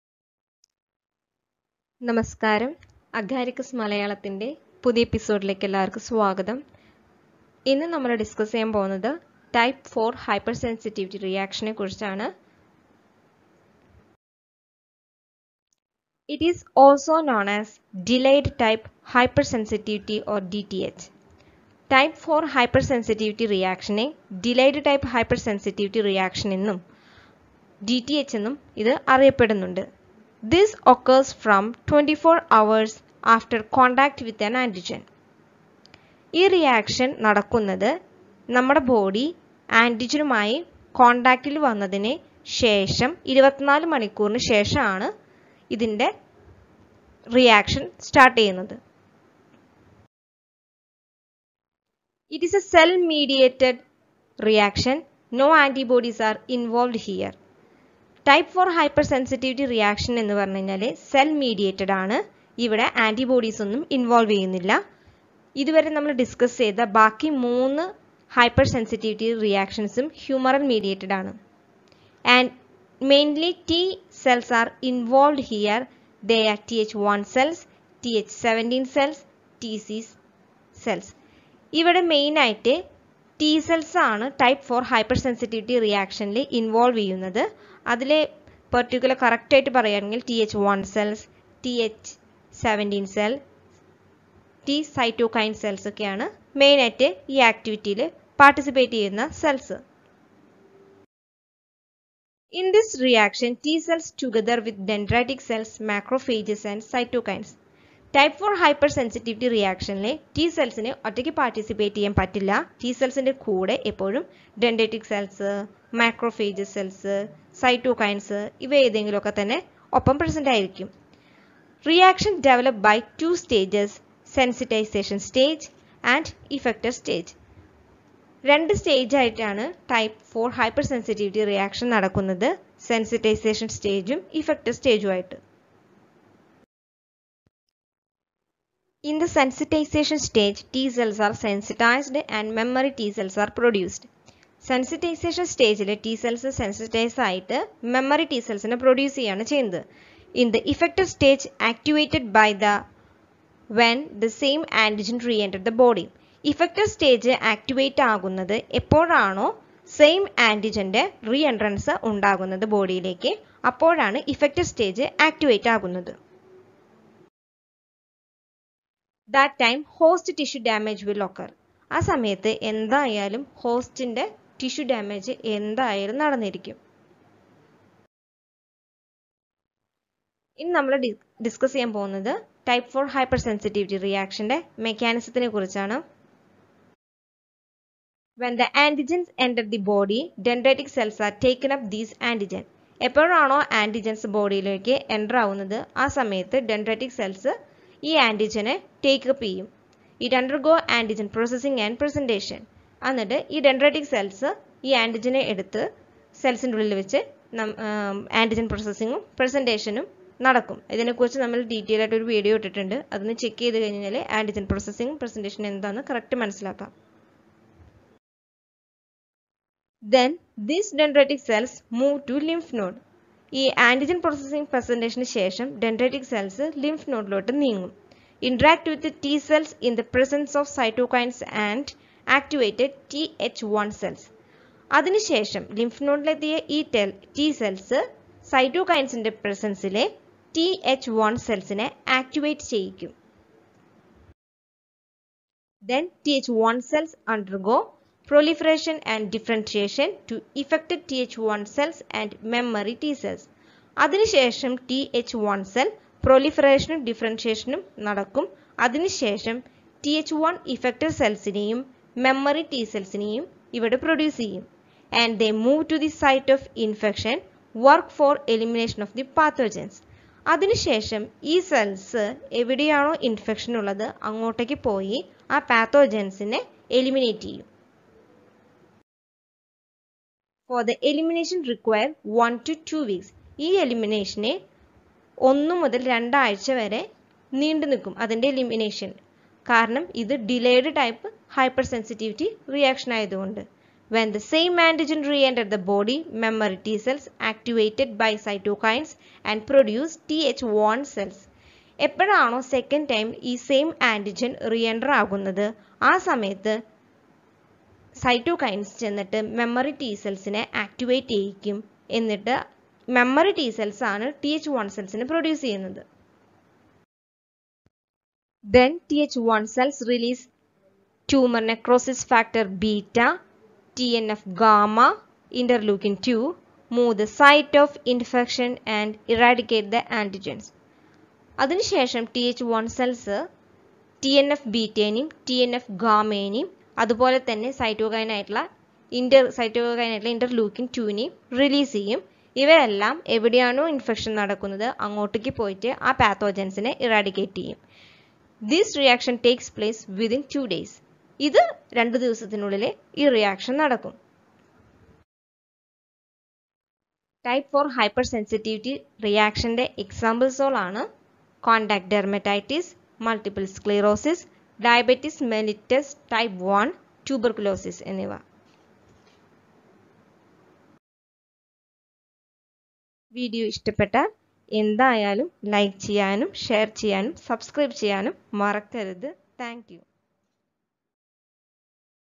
TYPE IV HYPERSENSITIVITY REACTIONS |DELAYED TYPE HYPERSENSITIVITY | DTH | MALAYALAM
Type IV hypersensitivity reactions or DTH  are cell mediated reactions.